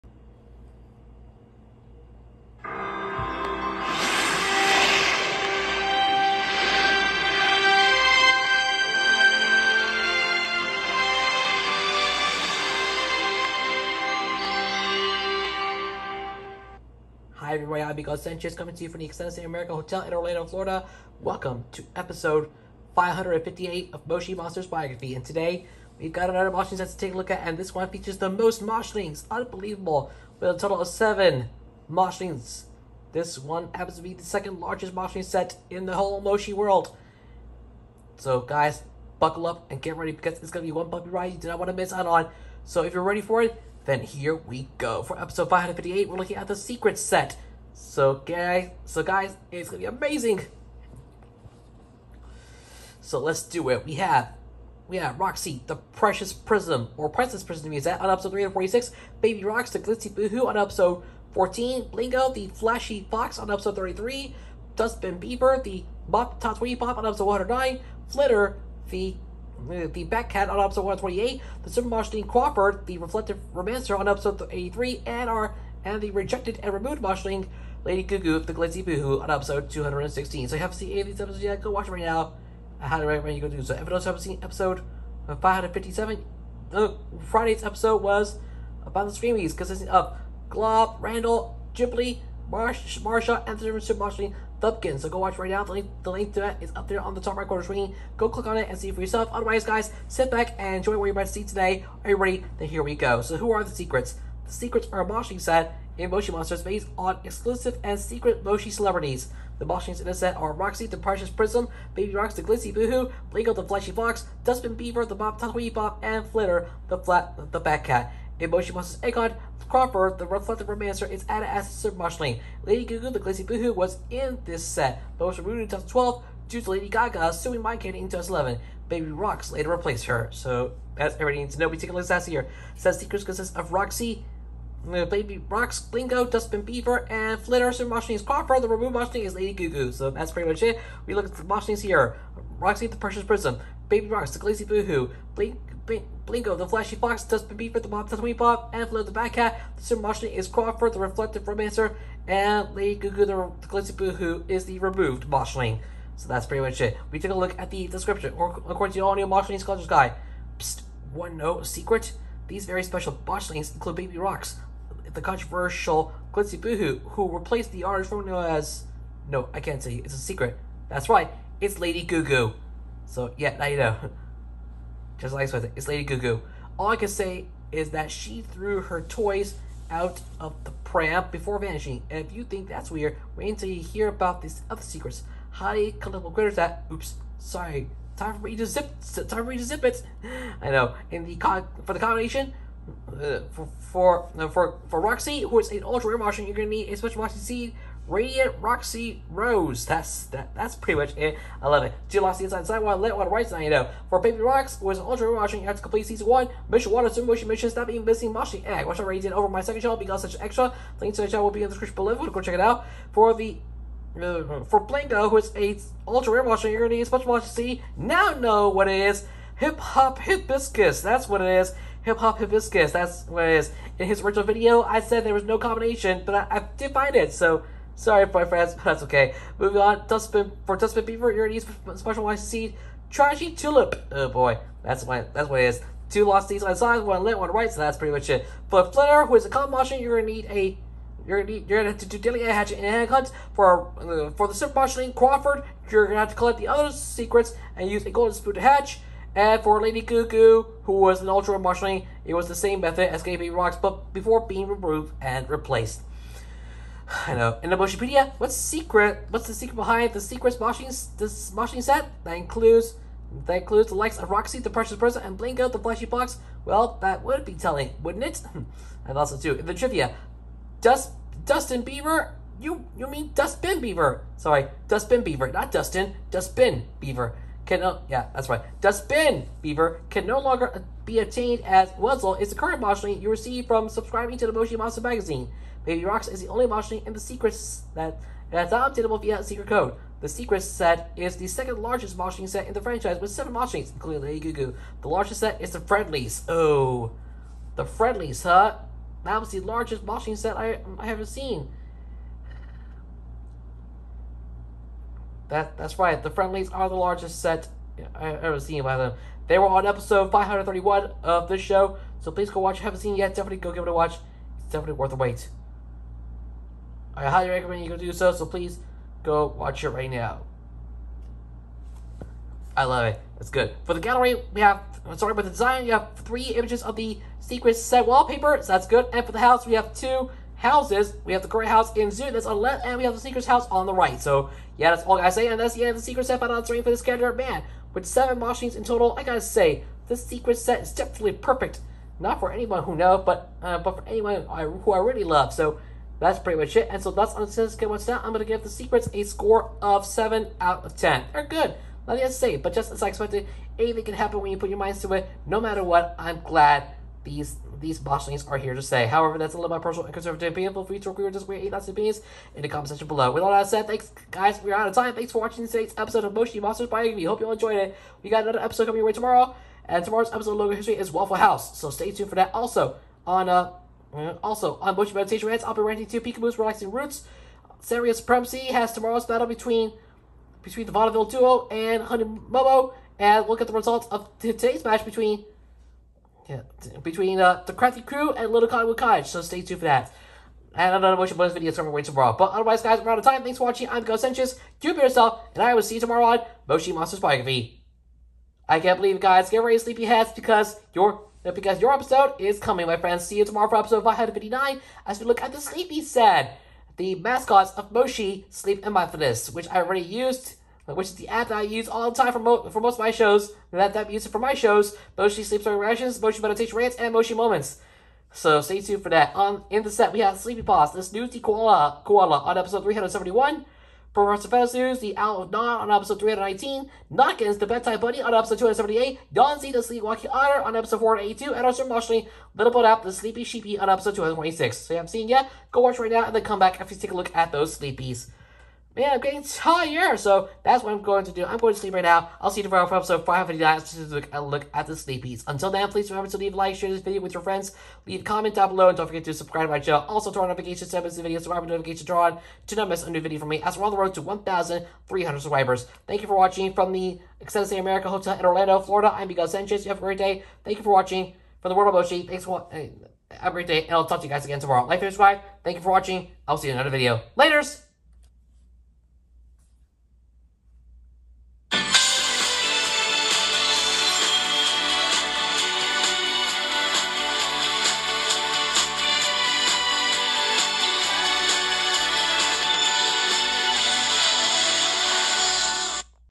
Moshi Monsters Biography #558 - Secrets
Hello, YouTube! Welcome to Episode 558 of Moshi Monsters Biography! In this episode, We take a look at the biography of the Secrets! Enjoy, guys!!